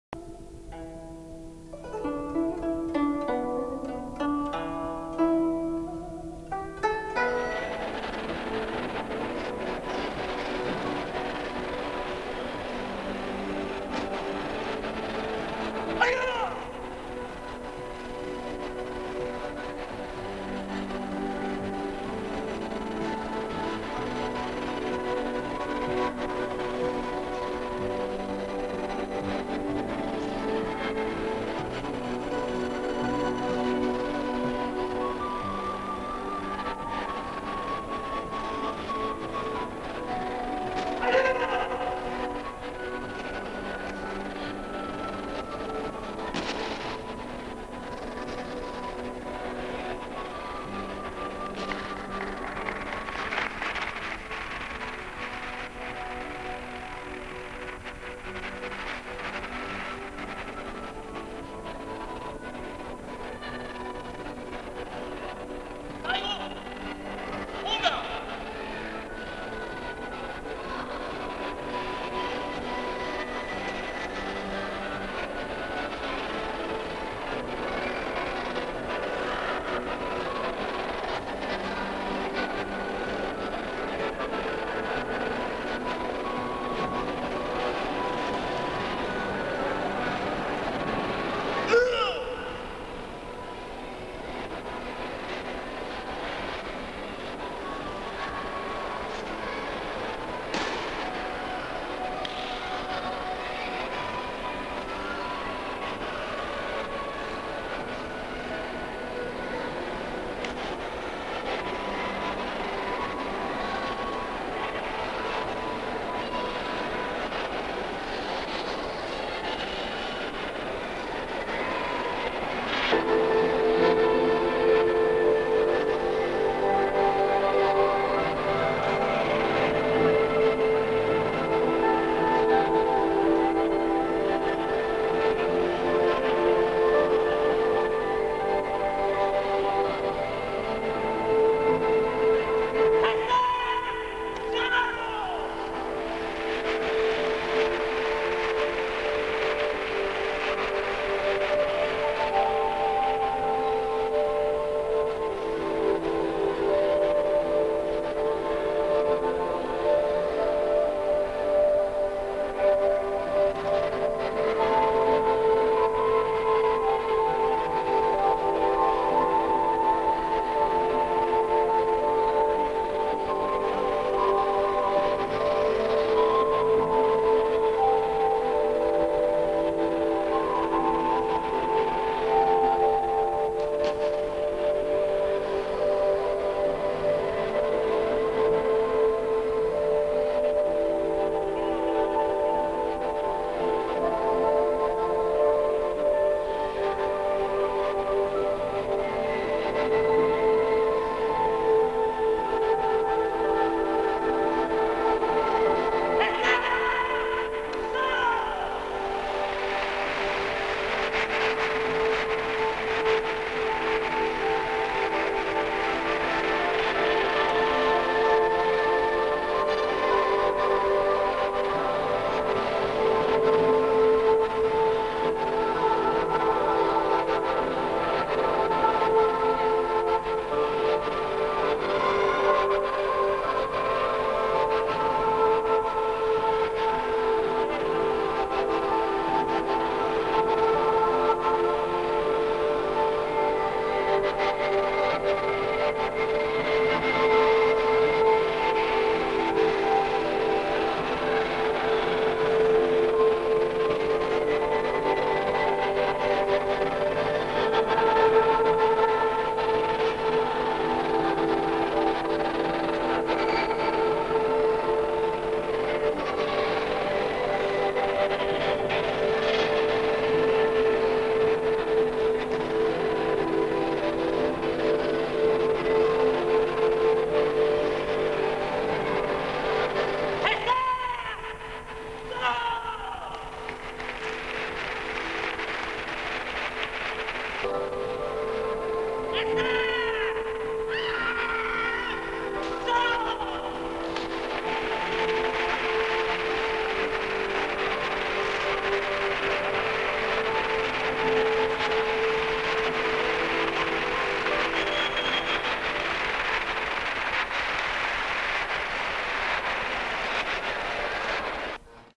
Exhibicion Taekwondo
I Exhibición de Artes Marciales de Mijas. Año 2.001, exhibición de Taekwondo por el Maestro Francis y el Maestro José Expósito Ríos "Maestro Abellana".